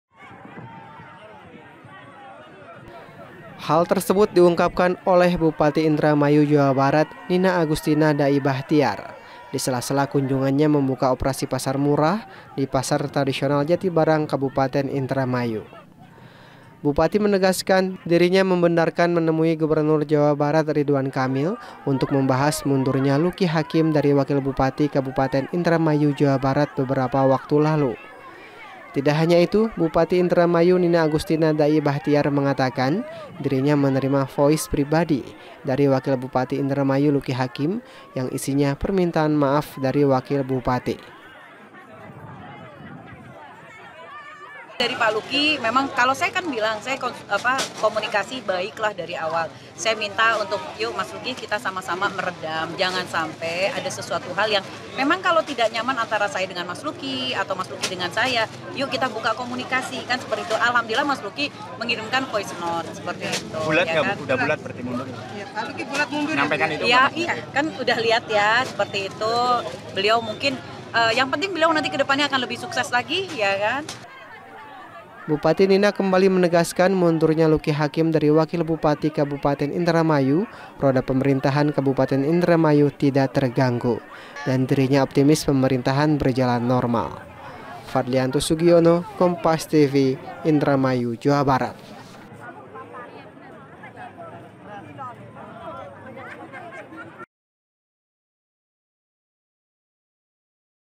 Roda Pemerintahan Tak Terpengaruh Mundurnya Lucky
INDARAMAYU.KOMPAS.TV, - Hal tersebut diungkapkan oleh Bupati Indramayu, Nina Agustina Da'i Bachtiar disela sela kunjungannya membuka operasi pasar murah di Pasar Tradisional Jatibarang Kabupaten Indramayu

Bupati menegaskan dirinya membenarkan menemui Gubernur Jawa Barat Ridwan Kamil untuk membahas mundurnya Lucky Hakim dari Wakil Bupati Kabupaten Indramayu beberapa waktu lalu

Tidak hanya itu Nina mengatakan dirinya menerima voice pribadi dari Wakil Bupati Indramayu Lucky Hakim yang isinya permintaan maaf dari Wabup.

Nina kembali menegaskan mundurnya Lucky Hakim dari Wakil Bupati Kabupaten Indramayu roda pemerintahan Kabupaten Indramayu tidak terganggu dan dirinya optimis pemerintahan berjalan normal.

Untuk lebih tahu berita terupdate seputarJawa Barat, bisa klink link di bawah .